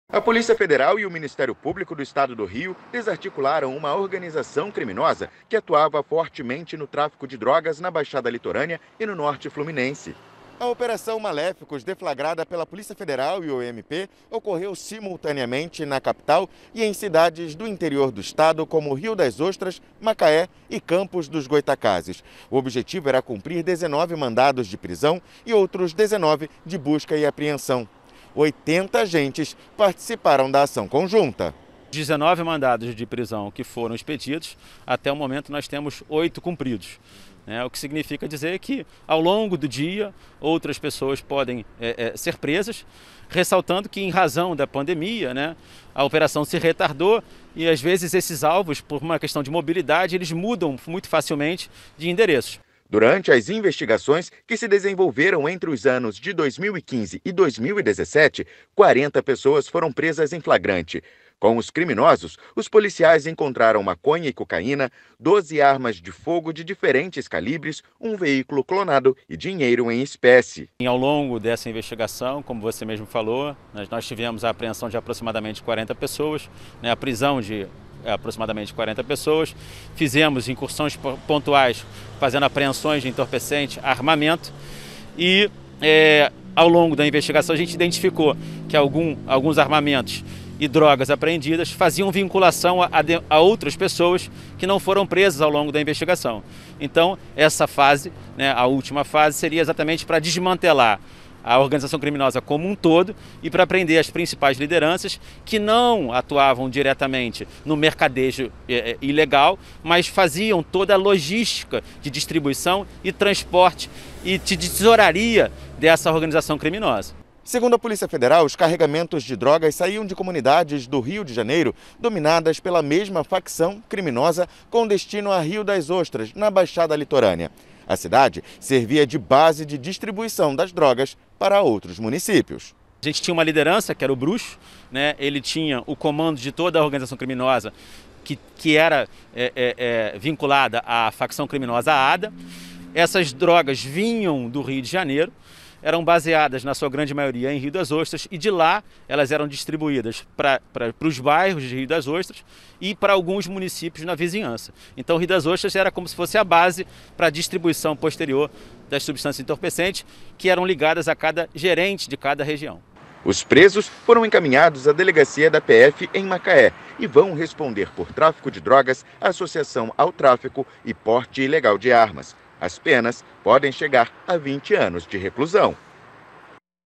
Operação contra o tráfico de drogas
Operação conjunta da Polícia Federal e o Ministério Público do estado do Rio de Janeiro para desarticular uma organização criminosa que atuava no tráfico de drogas no interior do estado.